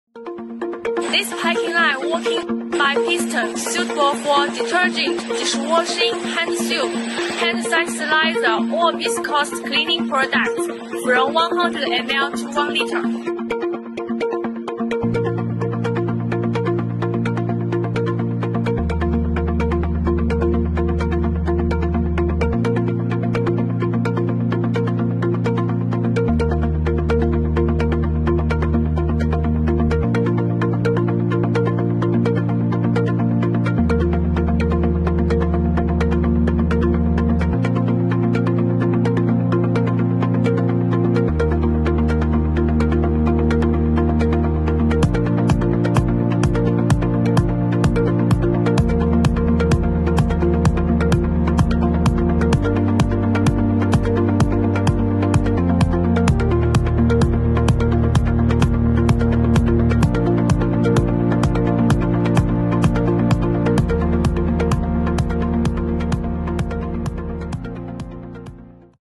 Liquid soap detergent filling machines packing line foamy Disinfectant antiseptic glass cleaner air
Liquid soap detergent filling machines packing line
Share for : viscous + foamy liquids
Disinfectant/ antiseptic/ glass cleaner/ air freshener…
Filling range : 250ml -5000ml
Capacity speed to choose : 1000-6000BPH
From :ZT-PACK